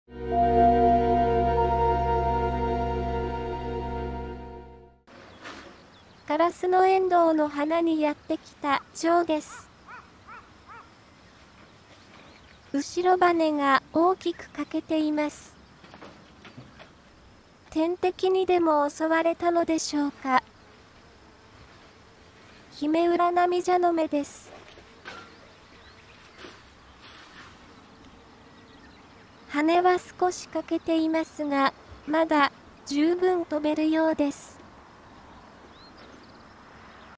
30秒の心象風景24615・翅が欠けても～ヒメウラナミジャノメ～
カラスノエンドウの花にやって来た チョウです。
後翅が大きく欠けています。
天敵にでも襲われたのでしょうか。
ヒメウラナミジャノメです。
翅は少し欠けていますが,まだ,充分飛べるようです。